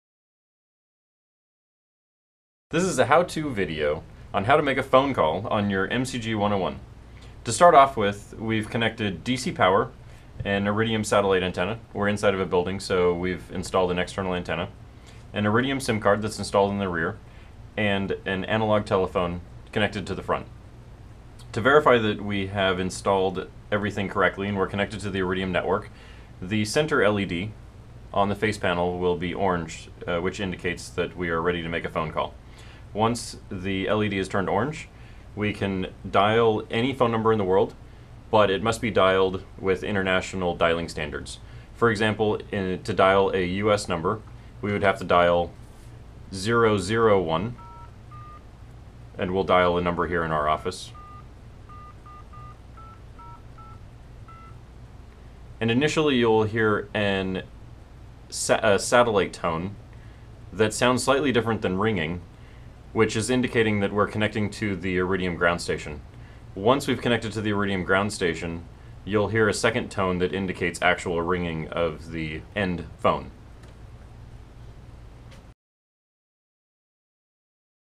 GSE MCG-101: How to make an Analog Phone Call
Learn how to use an analog phone to make a satellite phone call.